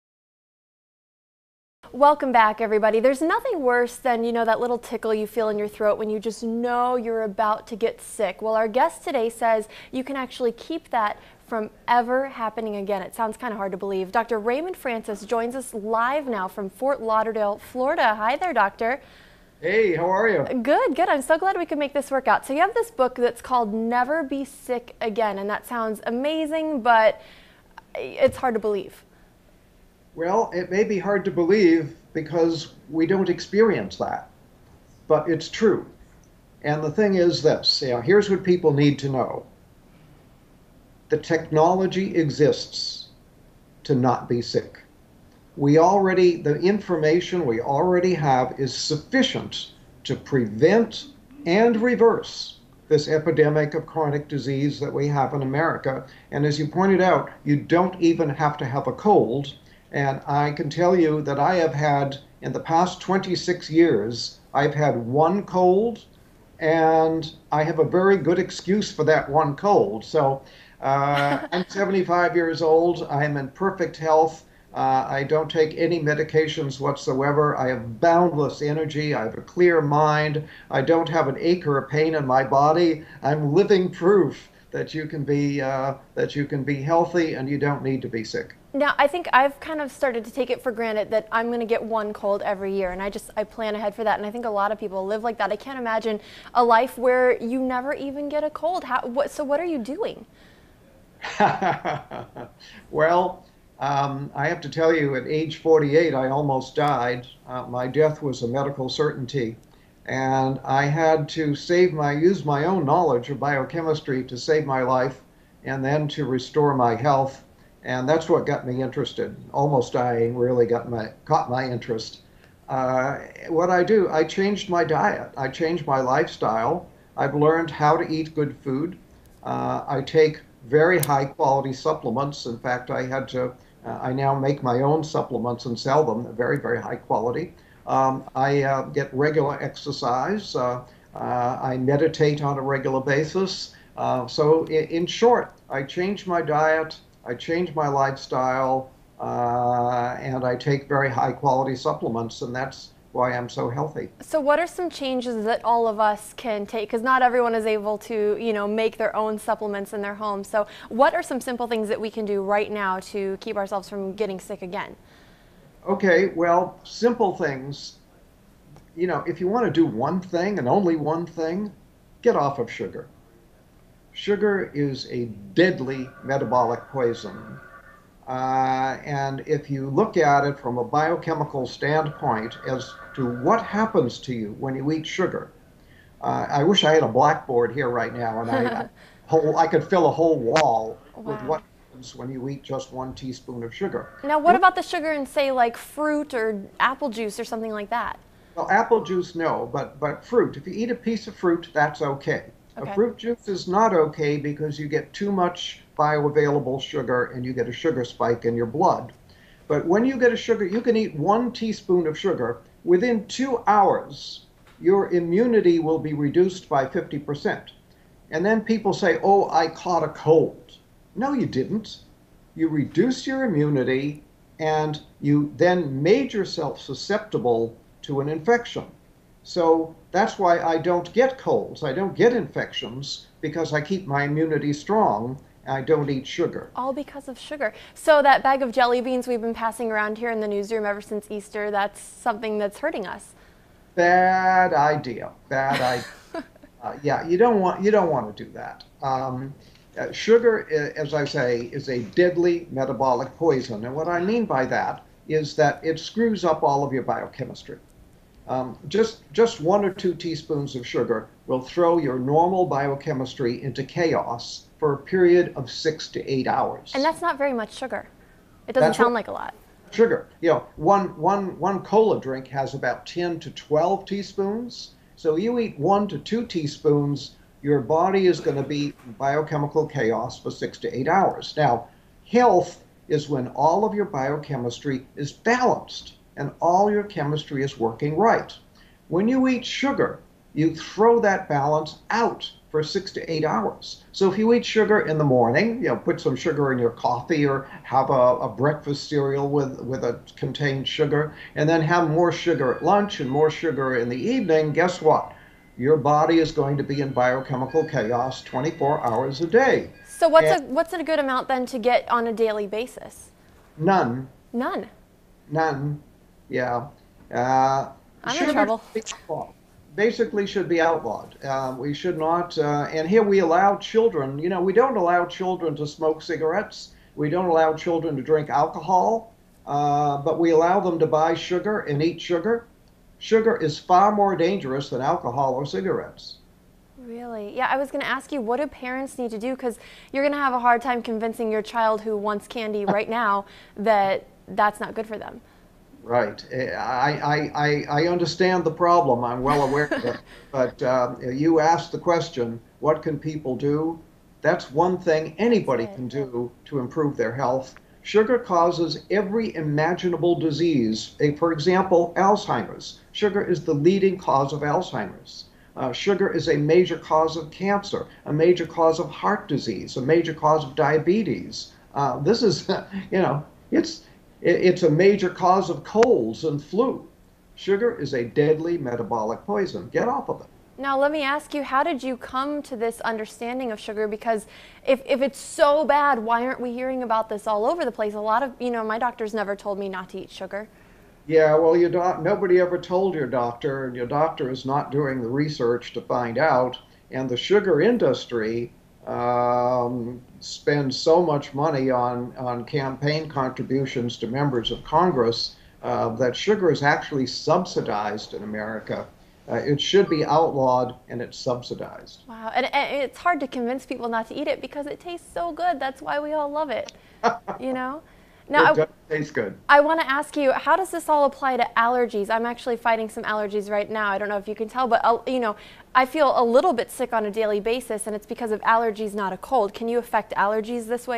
Interview of "Never be Sick again" author
Dr. Raymond Francis , Author of "Never be Sick again" book giving interview and interesting perspective that can be followed by everyone

Source: Whatsapp

The views and nutritional advice expressed are not intended to be a substitute for conventional medical service. If you have a severe medical condition or health concern, see your physician.